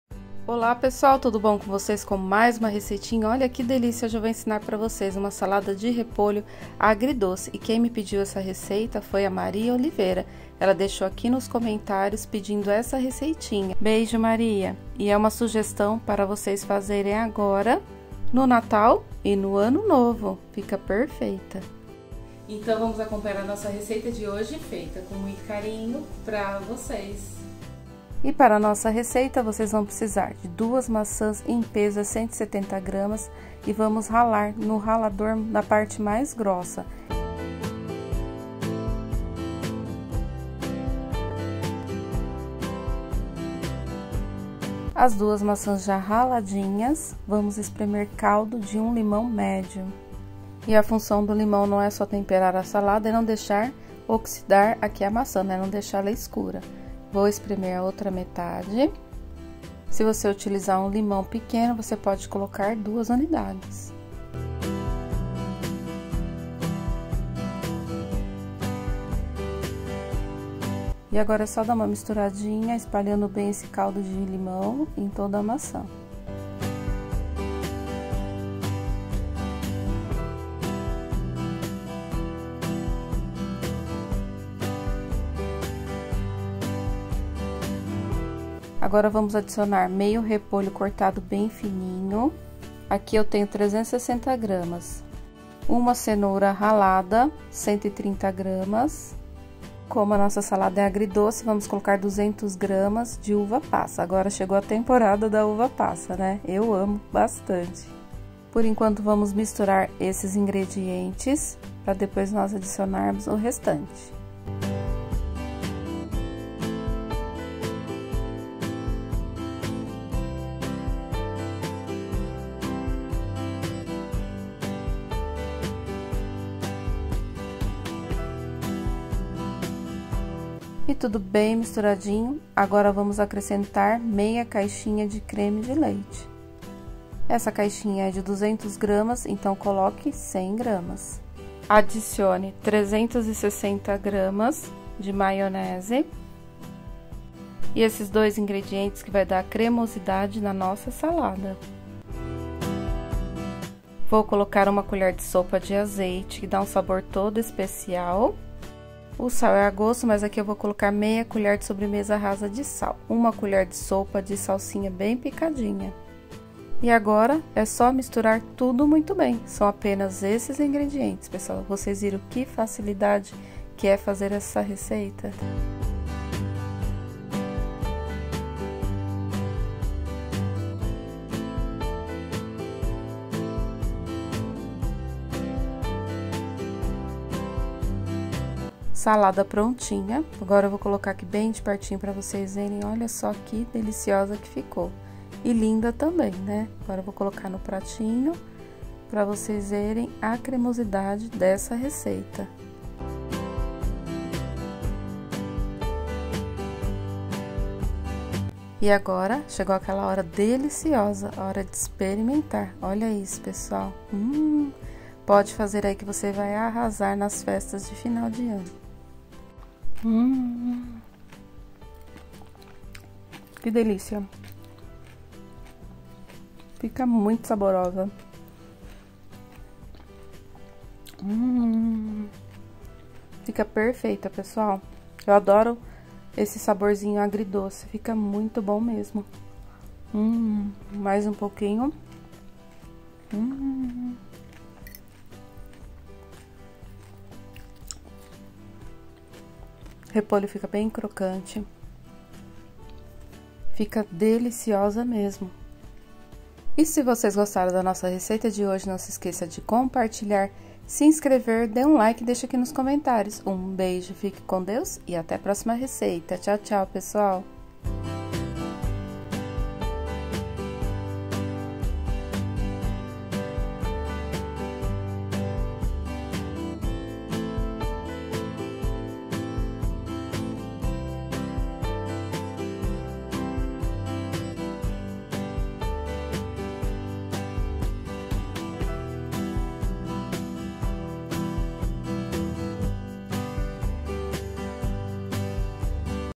SALADA DE REPOLHO AGRIDOCE DELÍCIOSA , PARA SEU NATAL E ANO NOVO !
INGREDIENTES
2 maçãs (170 Gr )  usei a gala mas pode ser a rua preferência
360 Gr de repolho
130 Gr de cenoura ralada
200 Gr de uva passas
1/2 colher de  sobremesa rasa de sal
1 colher de sopa cheia  de salsinha picadinha
1/2 caixinha de creme de leite ( 100 Gr )
360 Gr de maionese
Modo de preparo com o  passo a passo assista o vídeo completo obrigada 🥰 😘